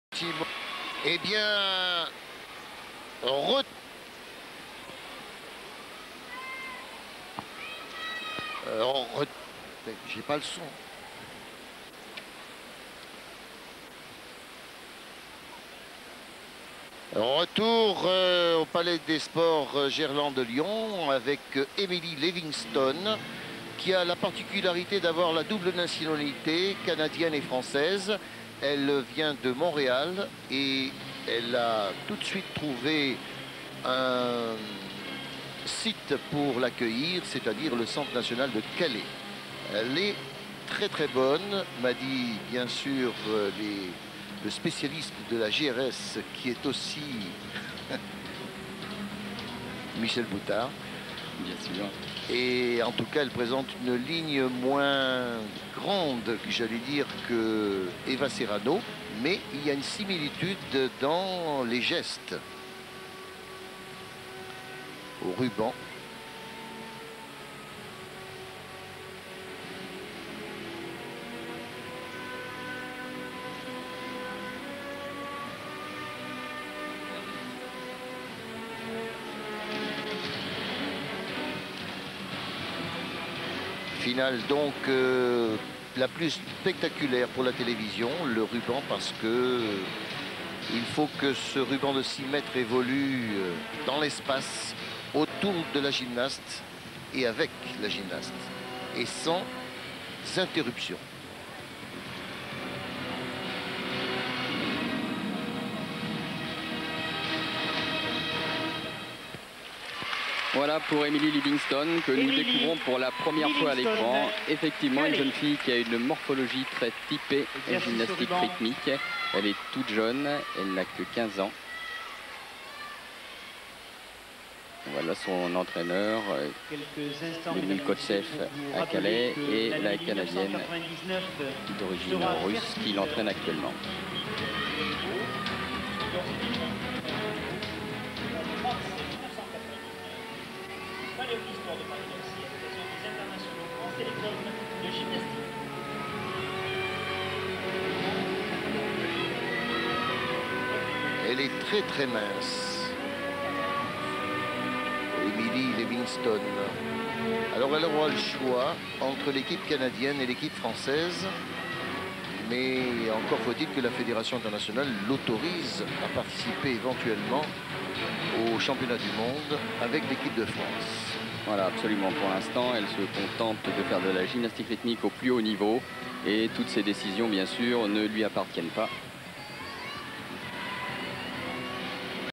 Emilie LIVINGSTON ruban - 1998 Championnats de France EF
Emilie LIVINGSTON - Finale ruban

Championnats de France '98 à Lyon

Note : 9.950